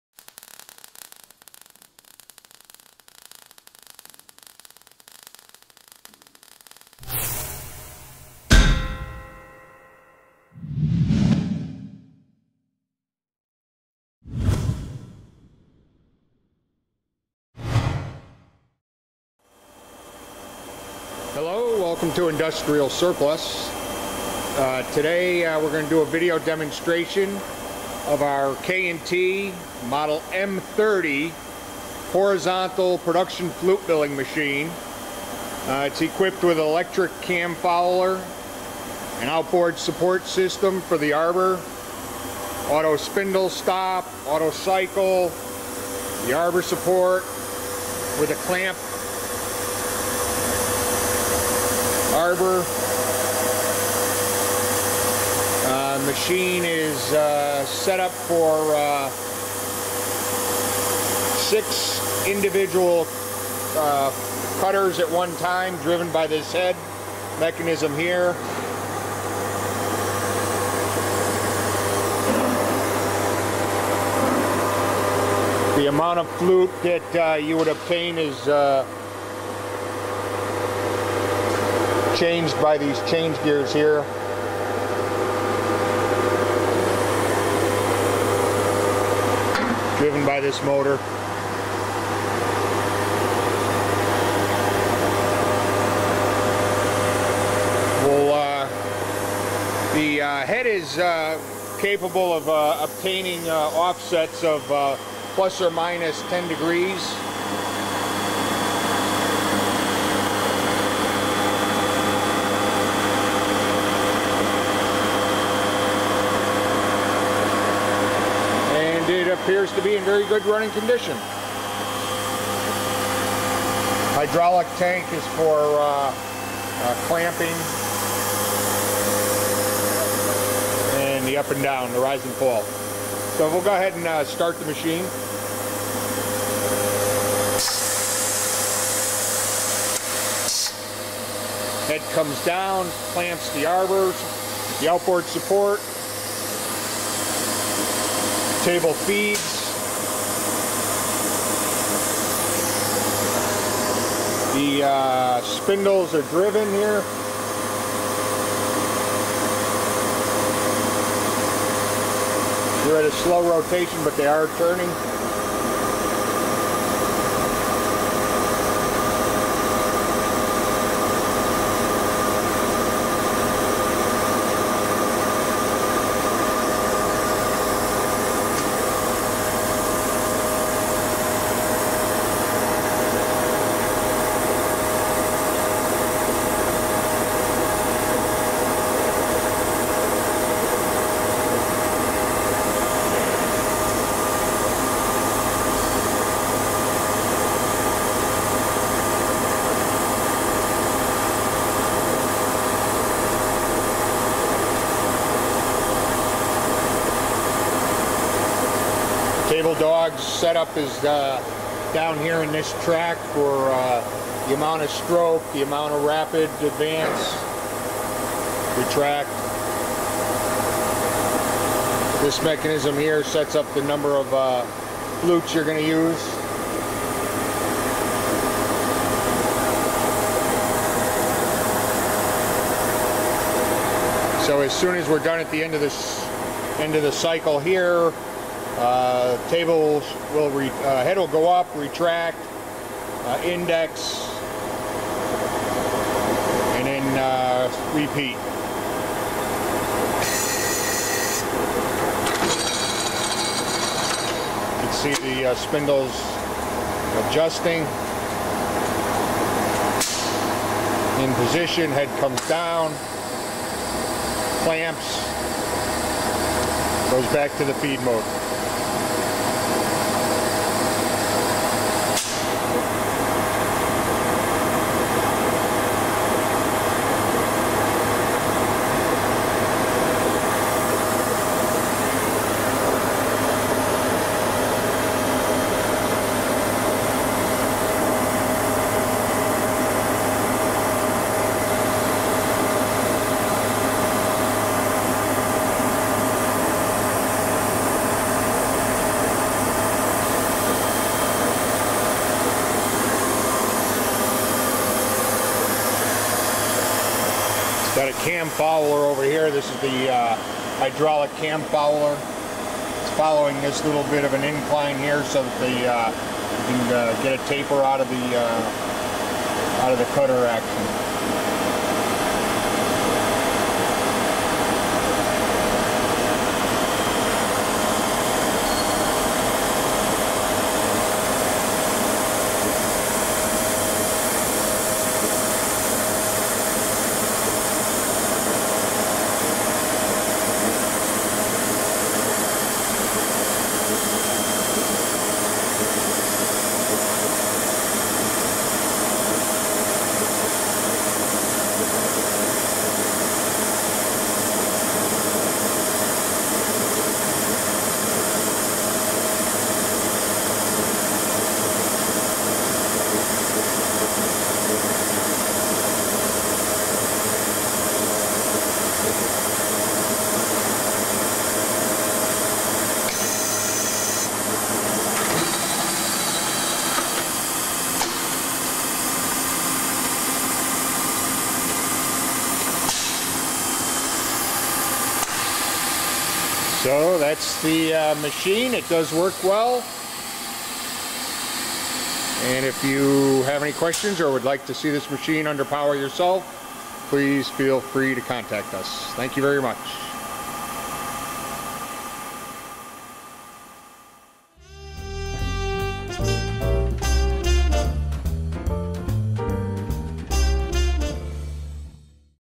Kearney & Trecker Universal Horizontal Flute Mill, Ref.#77-321 - SOLD
Kearny & Trecker Universal Horizontal Flute Miller    Ref.#77-321
Model No. M-30
Serial No. 8-6741

This is equipped with a universal swiveling head and a power take-off mechanism driving a 6-position rotating device which allows the automatic milling of straight or spiral flutes.

Table Size: 14-1/2" x 37"
Max. Distance Between Tailstock & Headstock: 27"
Center Distance Between Spindles: 2-1/2"
Center Height Over Table: 4-3/4"
Milling Head Swivel, Right to Left: 10 Deg.

Equipped With:
Automatic Rise & Fall of Milling Heads
Electric Cam Follower
Outboard Support
Auto Spindle Stop
Arbor Support
Auto Reverse
Auto Cycle